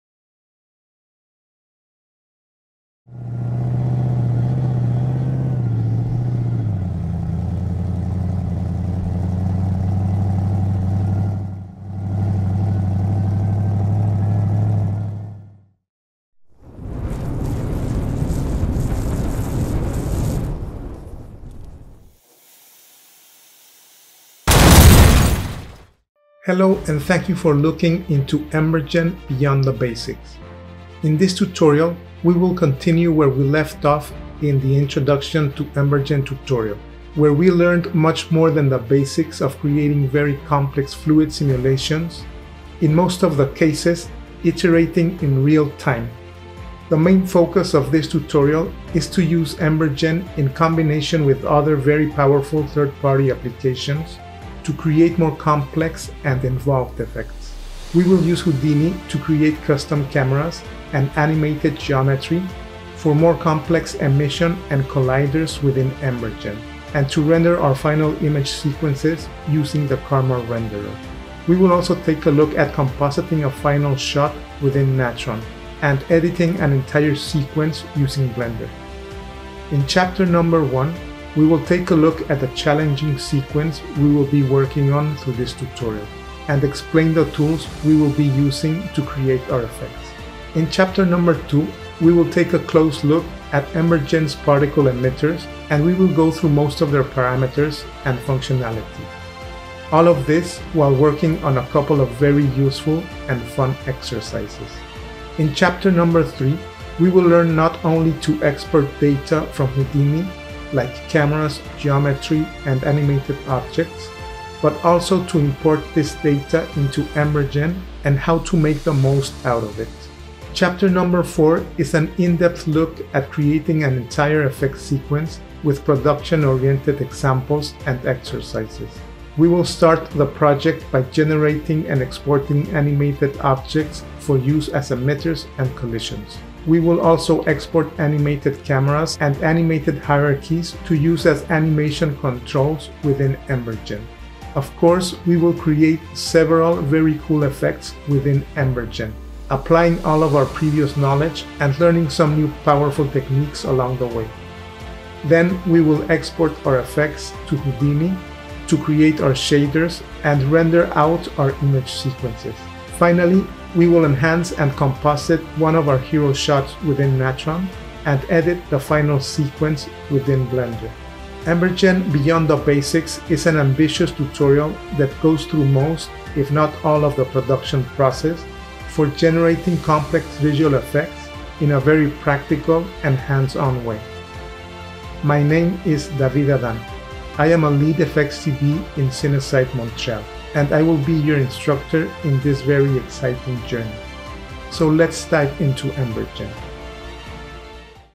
[TRAILER] EmberGen and Houdini: Advanced Techniques for Real Time VFX
Author: David Adan

This second course in the series offers an in-depth exploration into the advanced functionalities of EmberGen in combination with other third party software to create more complex effects. Participants will use Houdini to create geometry for complex and animated emissions and colliders, render the final images using the Karma renderer, and composite the final shot using Natron, a powerful compositing tool comparable with Nuke and other industry standard software.

Designed for those with foundational EmberGen skills, this course empowers artists to execute sophisticated visual effects for gaming and film projects with confidence.

Lesson Breakdown

Introduction and Overview: Understand the tools and techniques we’ll use to create a challenging VFX sequence.

Particle Emitters: Explore EmberGen’s particle emitters, parameters, and functionalities through practical exercises.

Data Integration: Learn how to export and import cameras, geometry, and animated objects between Houdini and EmberGen.

Full Effect Sequence: Follow along as we create, animate, and export complex effects, apply shaders, and render image sequences using Houdini’s Karma renderer.

You’ll wrap up with compositing in Natron and editing in Blender, ensuring you have a comprehensive understanding of the entire production process.

Key Takeaways
Good understanding of how to export geometry and cameras from Houdini for use in EmberGen.
Exporting data from EmberGen for rendering in other third-party applications like Karma.
Creating advanced fluid simulations and effects in EmberGen for rendering in professional visual effects software.

Target Audience
Visual effects artists with basic to intermediate EmberGen experience.
Professionals seeking to integrate EmberGen with other VFX tools.
Artists aiming to enhance their skills in creating complex effects for games and visual projects.

Prerequisites
Viewing the FREE course "Introduction to Embergen: Real-Time Fluid Simulation for VFX" is highly recommended for complete beginners to the JangaFX software.
Familiarity with Houdini and basic visual effects concepts.
Basic Experience with compositing tools like Natron or similar software.

Key Benefits of Using EmberGen and Houdini Together:

Seamless Integration: Combining EmberGen with Houdini allows for seamless integration of particle simulations and geometry, enhancing the overall efficiency and effectiveness of the VFX creation process.

Advanced Fluid Simulations: Utilize Houdini’s powerful tools for creating complex geometries and animations, and then leverage EmberGen’s real-time capabilities to generate stunning fluid simulations quickly.

Enhanced Rendering Capabilities: Houdini's Karma renderer, paired with EmberGen's VDB exports, provides high-quality, photorealistic renders of simulations, ensuring that the final output is both visually impressive and professionally polished.

Versatility in Data Management: The ability to export and import various file formats (such as Alembic and VDB) between EmberGen and Houdini allows for a flexible workflow that can accommodate different project requirements and pipelines.

Efficient Workflow: Using EmberGen's real-time simulation capabilities in conjunction with Houdini's robust feature set can significantly speed up the iteration process, allowing artists to see the results of their adjustments immediately and make necessary changes on the fly.

Comprehensive Toolset: The combination of these two tools provides a comprehensive suite for VFX artists, enabling them to handle everything from particle emission and collision to final rendering and compositing within a cohesive workflow.